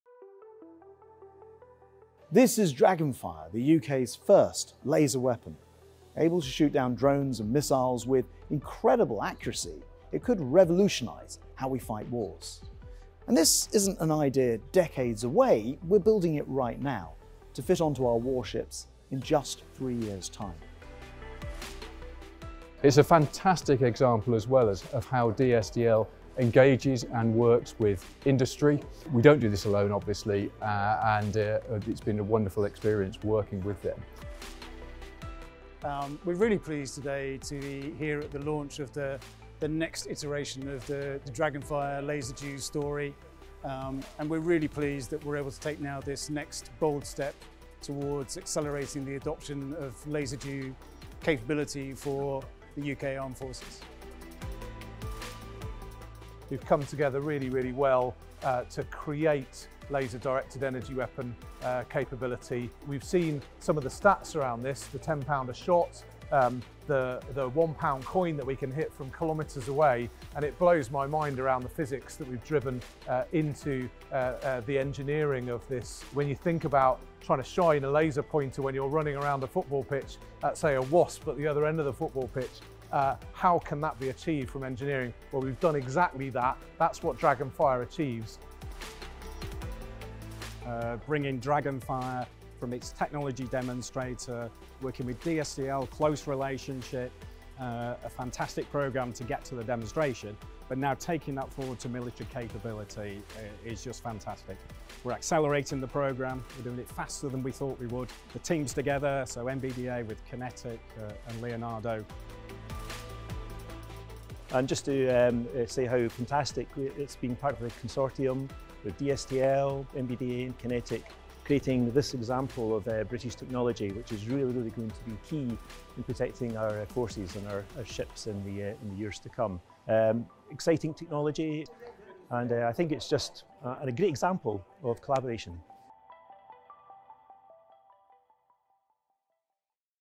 DragonFire: The Team Behind The Beam
The cutting-edge DragonFire laser will be installed on Royal Navy warships for the first time from 2027 – far quicker than previously envisaged thanks to new defence procurement model.

Able to fire at any target visible in the air at around £10 a shot and with an accuracy equivalent to hitting a pound coin from a kilometre away, the powerful DragonFire weapon will provide highly effective defence against drone and missile threats.